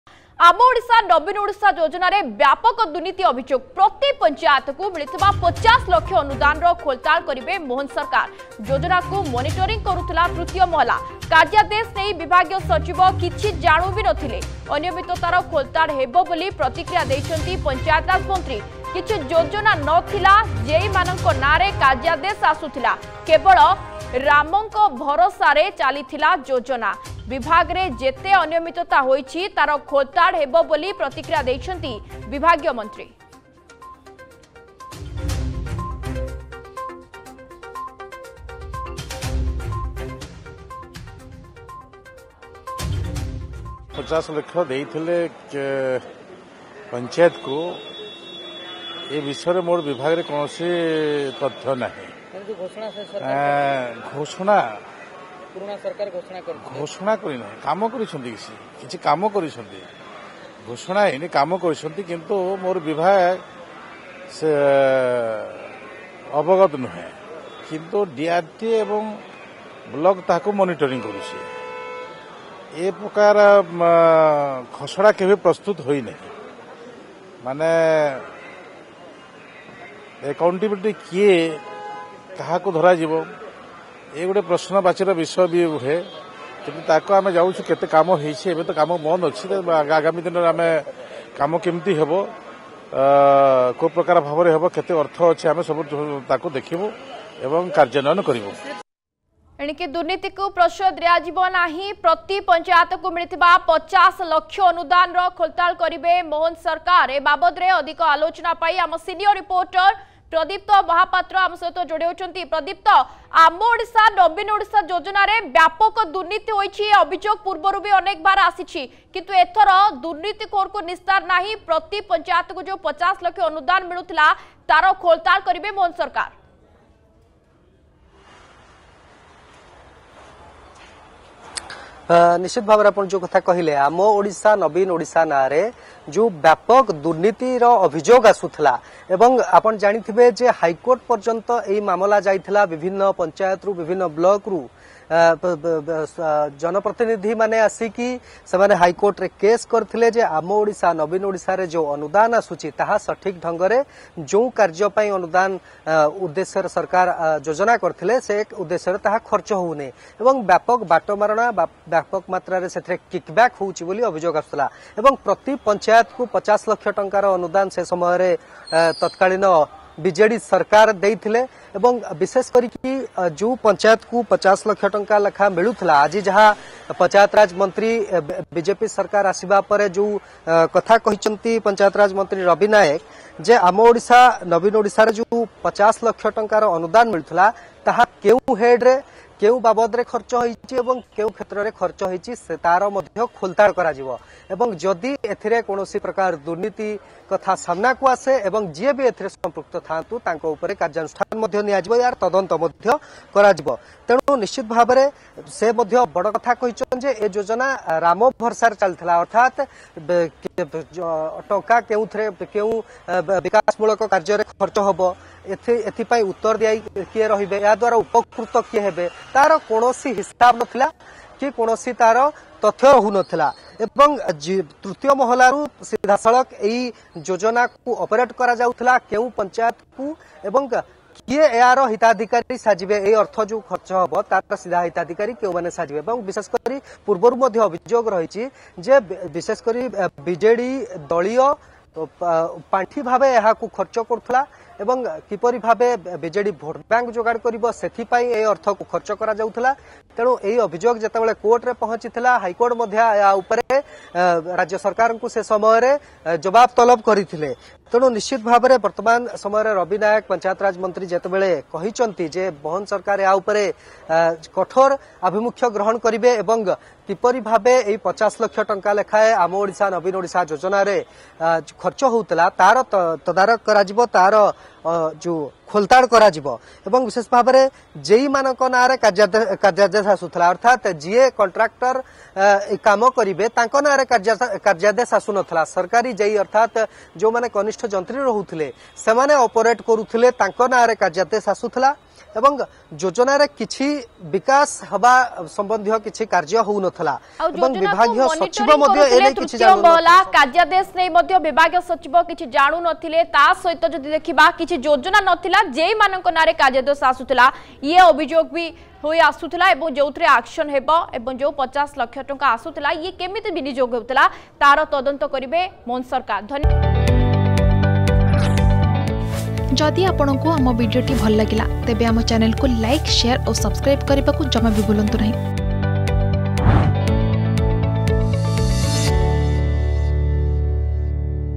`୫୦ ଲକ୍ଷ ଅନୁଦାନର ଯାଞ୍ଚ ହେବ' || Ama Odisha Naveen Odisha
ଆମ ଓଡିଶା ନବୀନ ଓଡିଶା ଯୋଜନାରେ ବ୍ୟାପକ ଦୁର୍ନୀତି ଅଭିଯୋଗ । ପ୍ରତି ପଂଚାୟତକୁ ମିଳିଥିବା ୫୦ ଲକ୍ଷ ଅନୁଦାନର ଖୋଲତାଡ କରିବେ ମୋହନ ସରକାର । ଯୋଜନାକୁ ମନିଟରିଂ କରୁଥିଲା ତୃତୀୟ ମହଲା ।